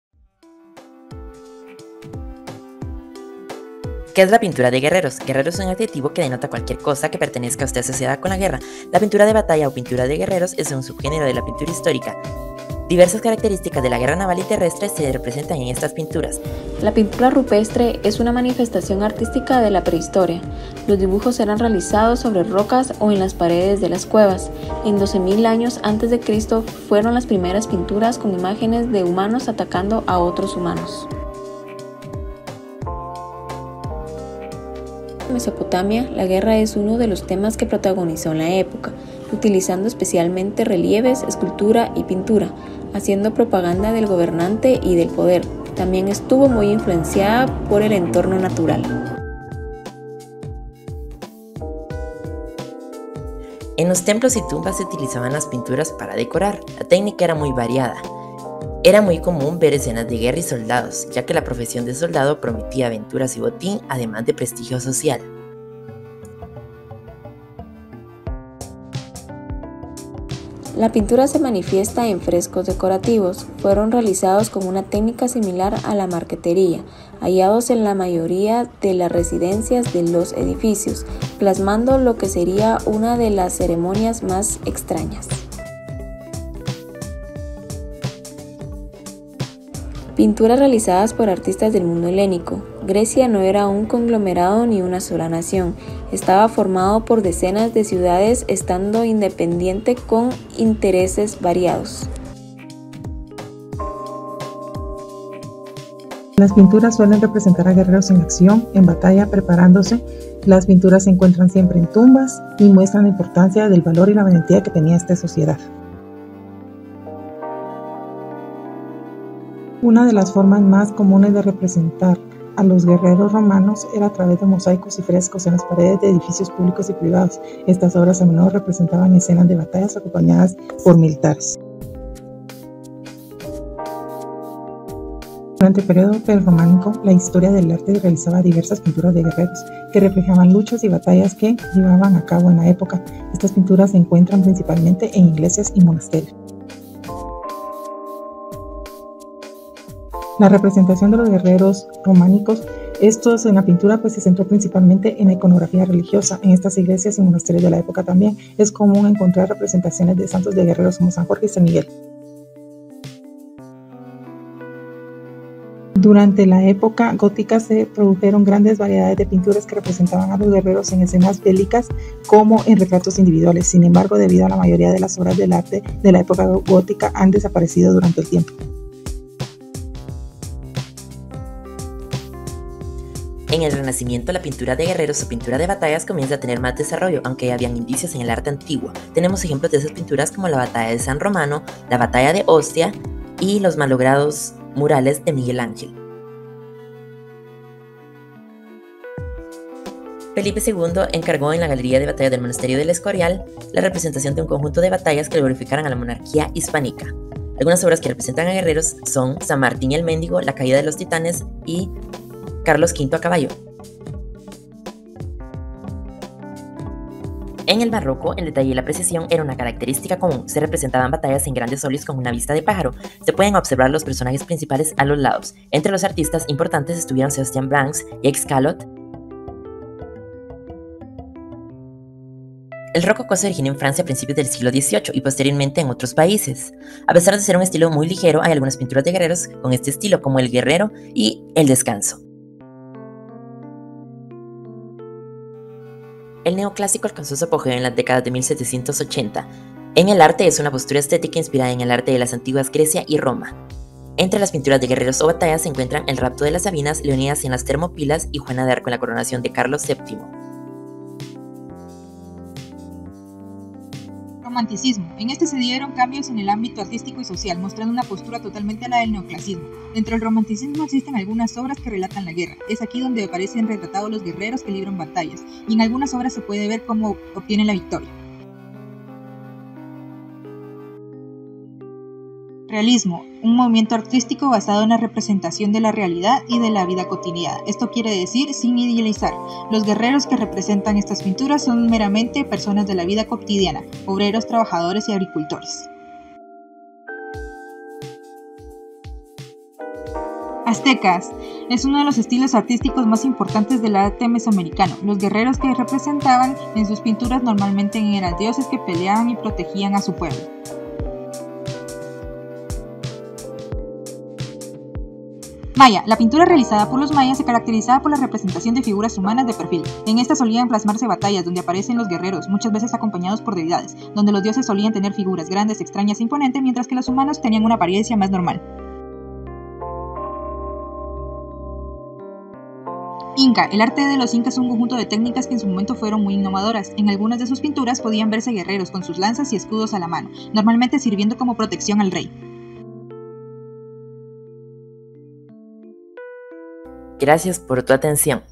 Pintura de Guerreros_Grupo 3_Sección F
Grupo 3 - AHAD1- Jornada Vespertina - Sección F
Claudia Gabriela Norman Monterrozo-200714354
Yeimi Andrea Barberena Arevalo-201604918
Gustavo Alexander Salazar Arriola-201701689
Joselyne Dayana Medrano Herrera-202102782

En este video aprenderás sobre la pintura de batalla o pintura de guerreros, este es un subgénero de la pintura histórica. Diversas características de la guerra naval y terrestre se representan en pinturas de paisajes y batallas típicas. Se pueden observar diferentes elementos como uniformes, armas y equipo, barcos, caballos, etc.

Proyecto académico realizado en la asignatura de Análisis Histórico del Arte y el Diseño Visual 1, año 2023. Las imágenes y audio utilizados son propiedad de sus autores y son utilizados para efectos académicos sin fines de lucro